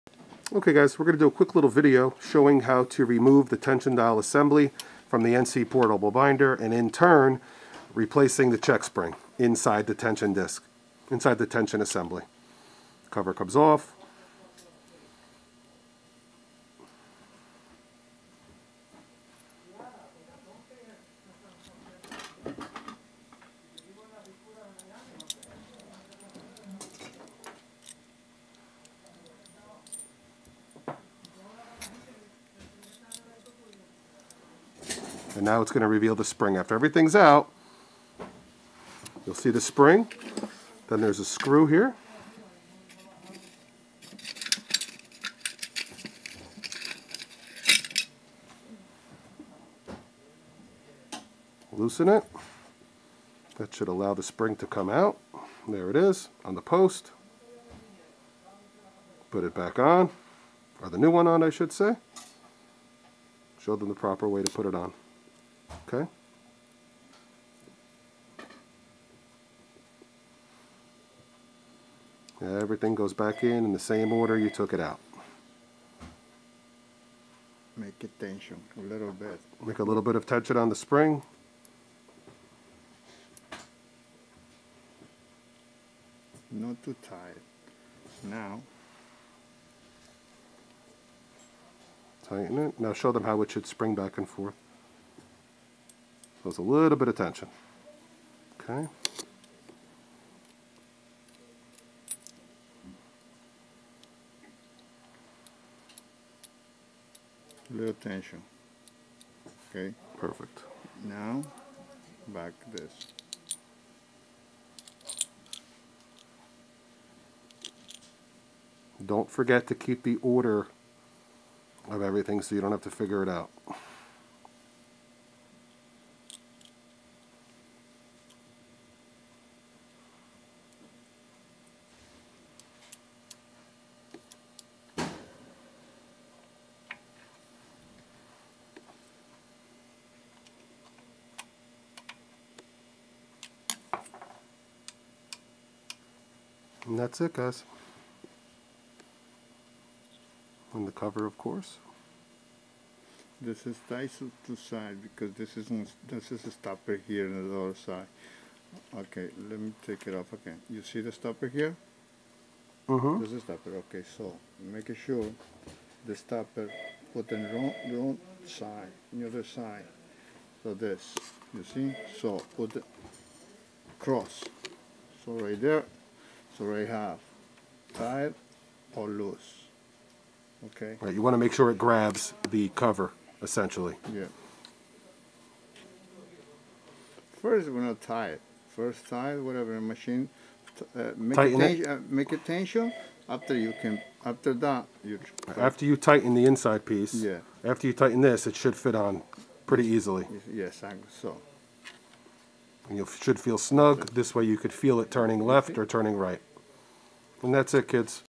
How to change the check spring in the tension dial for NC Carpet Binders model PBS & PBT. CS, PS76
How to change the spring in the tension dial for NC Carpet Binders model PBS & PBT.